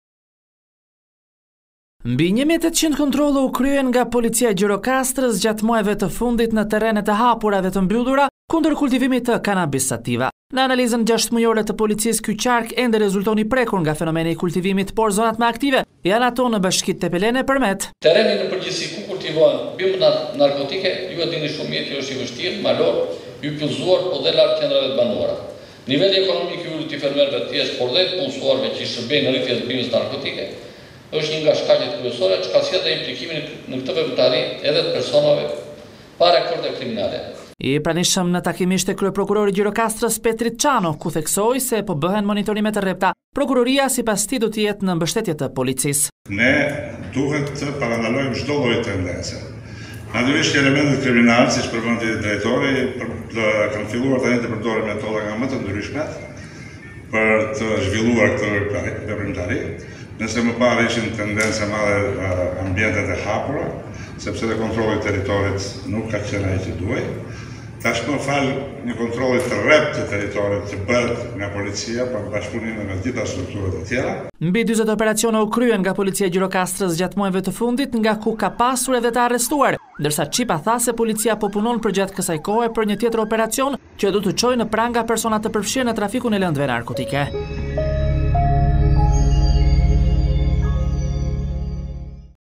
Në 6 muaj 1800 kontrolle në Gjirokastër për parcela e kabanis!Zonat më aktive Tepelena e Gjirokastra
Pas 1800 kontrolleve nga policia, Qarku Gjirokastër nuk rezulton të jetë përfshirë në fenomenin e kanabizimit. Zonat më aktive mbeten Tepelena dhe Përmeti, ndërsa kryepolici Ardian Çipa kërkon bashkëpunim më të ngushtë institucional.